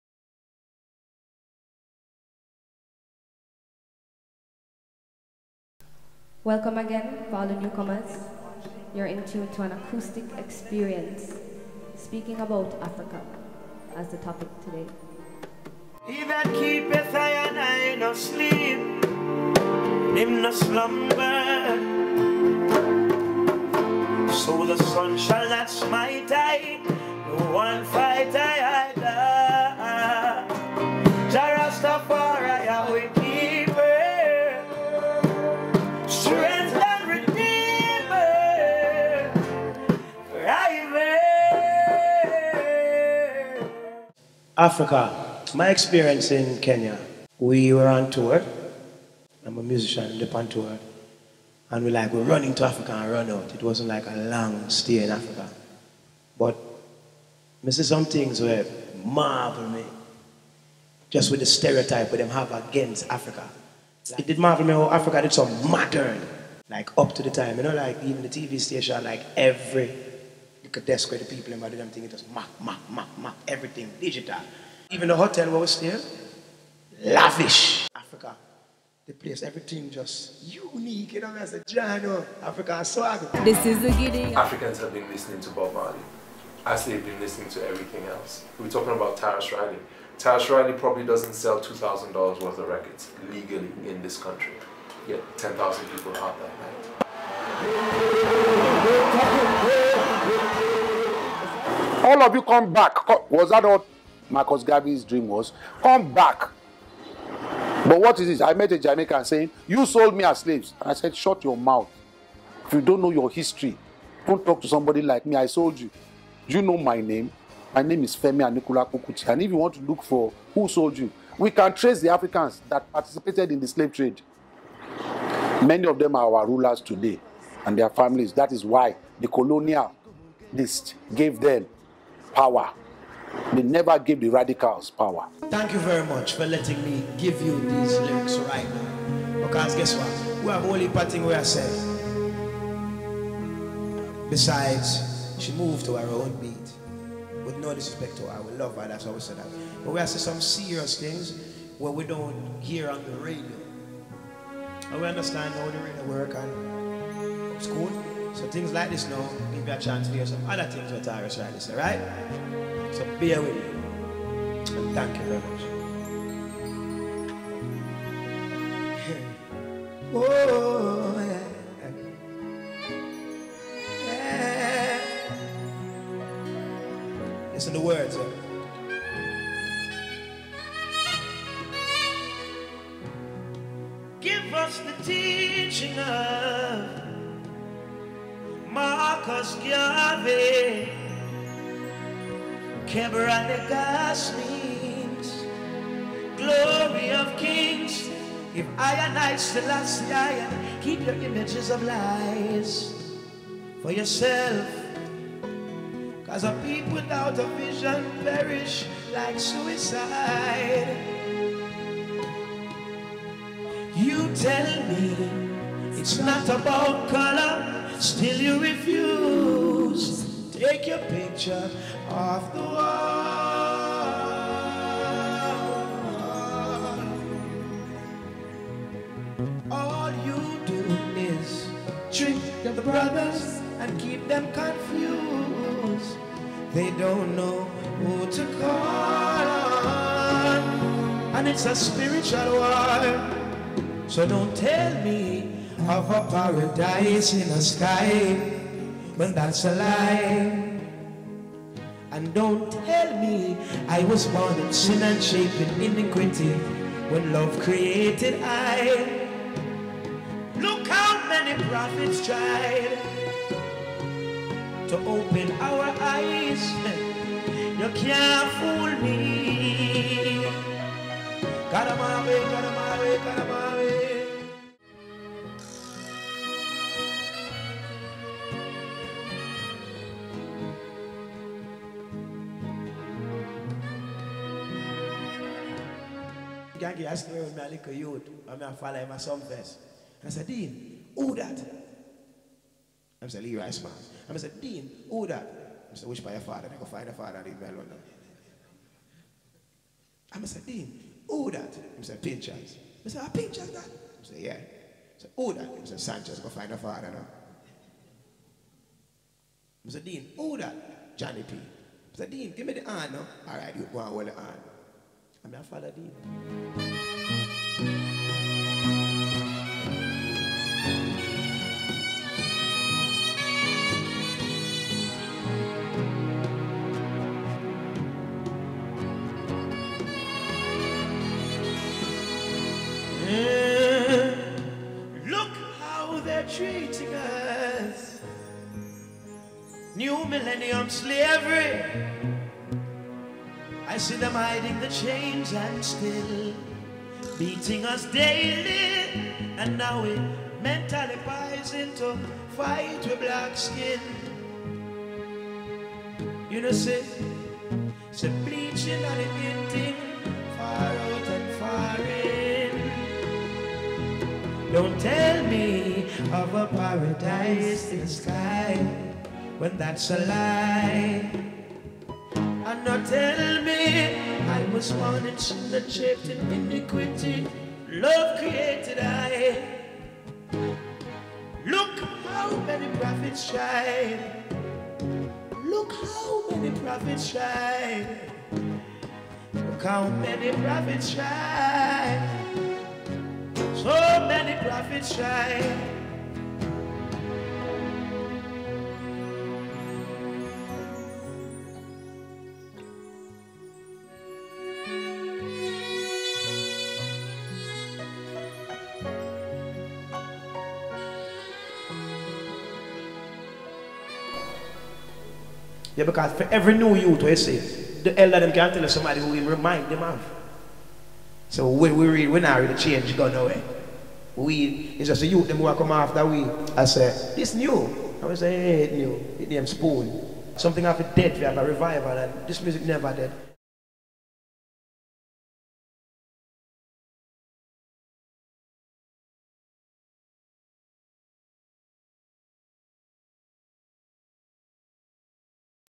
CONVERSATIONS WITH TARRUS RILEY
CONVERSATIONS is an Up Close and Acoustic Experience with different Artistes, Visionaries and Pioneers about their Art & Mission. May 2013 we had the opportunity to share time and space with Tarrus Riley with the music business & Afrika in Focus. Filmed in Tuff Gong Studios.